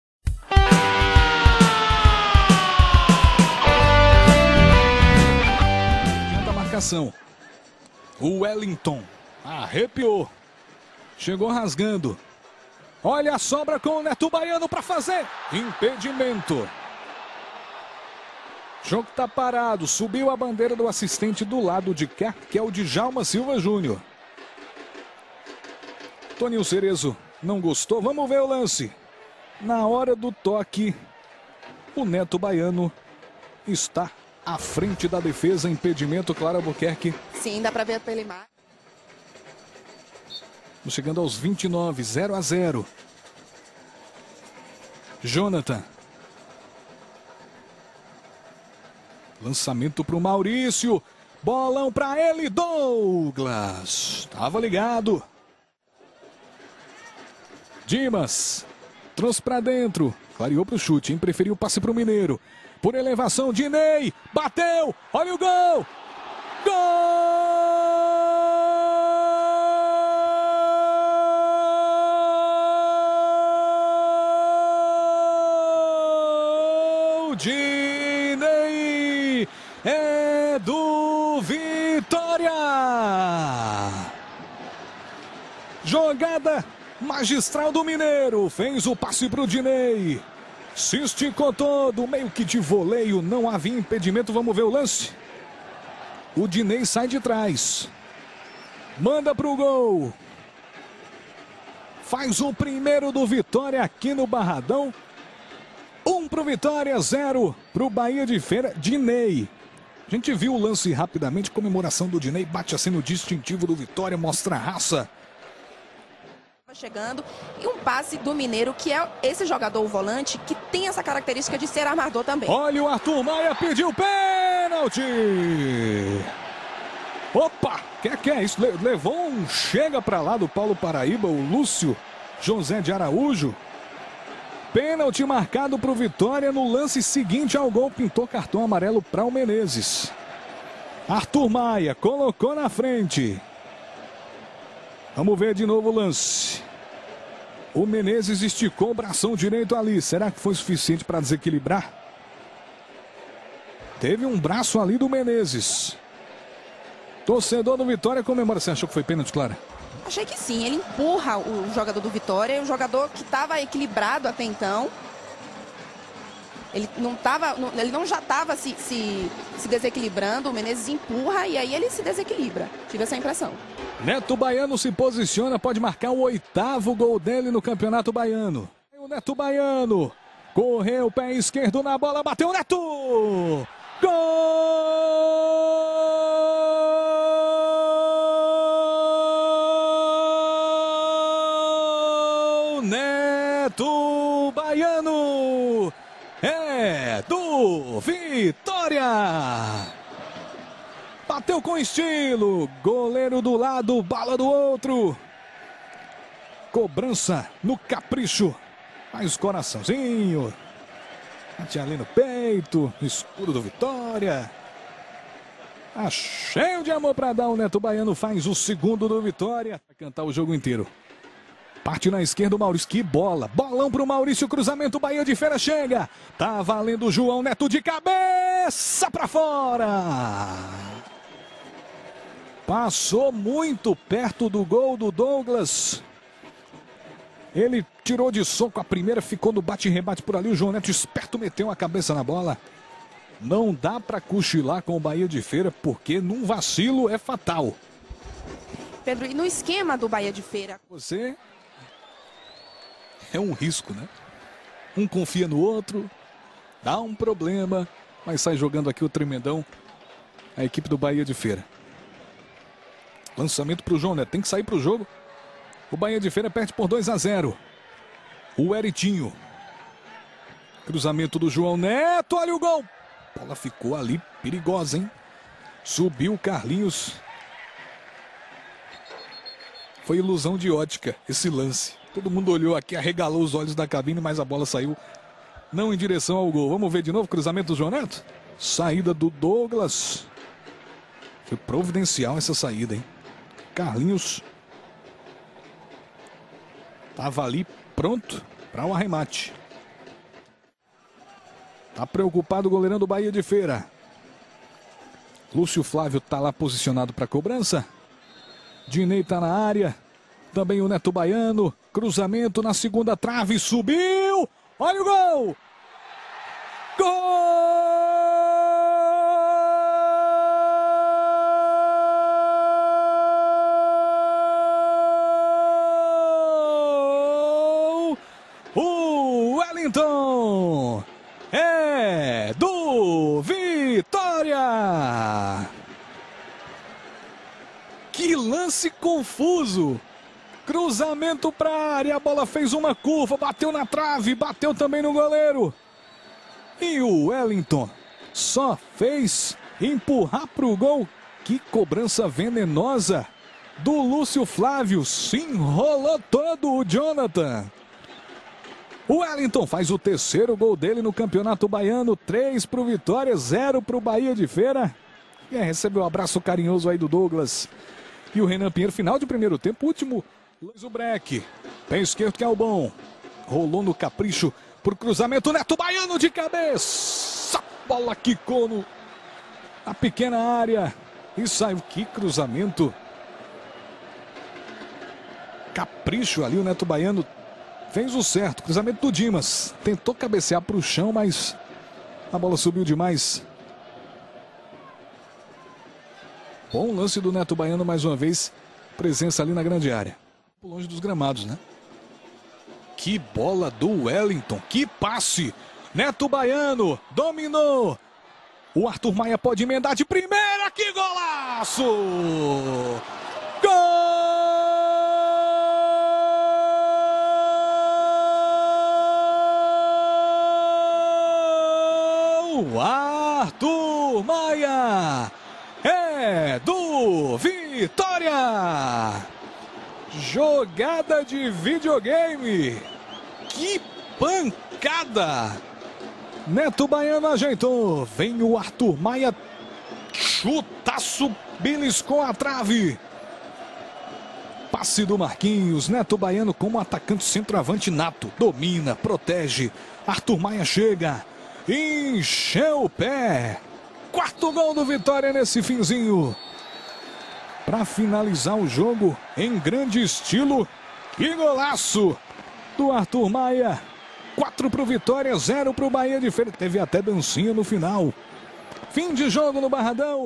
Vitória 4x0 bahia de Feira - 6ª rodada - Campeonato Baiano 2012
Vitória 4x0 Bahia de Feira - 6ª rodada do Campeonato Baiano 2012.

Local: Estádio Manoel Barradas (Barradão), em Salvador (BA).
Data: Domingo, 5 de fevereiro.
Horário: 17h.

Árbitro: Lúcio José Silva de Araújo.
Assistentes: Dijalma Silva Ferreira Júnior e Carlos Vidal Pereira de Oliveira.

Gols: Dinei, Neto Baiano, Uelliton e Arthur Maia (Vitória, aos 32, aos 35 e aos 48 minutos do primeiro tempo, e aos 46 do segundo, respectivamente).

Cartões amarelos: Geovanni e Léo (Vitória); Menezes, Paulo Paraíba e Weritinho (Bahia de Feira).

Vitória: Douglas; Léo, Gabriel Paulista, Alan Henrique e Wellington Saci (Dimas); Uelliton, Mineiro, Arthur Maia e Lúcio Flávio (Geovanni); Dinei (Marquinhos) e Neto Baiano. Técnico: Toninho Cerezo.

Bahia de Feira: Dionatan; Thiago Granja, Paulo Paraíba, Menezes e Weritinho; Carlos, Lau (Rômulo), Johnattan e Maurício (Juninho); João Neto e Carlinhos (Hugo). Técnico: Arnaldo Lira.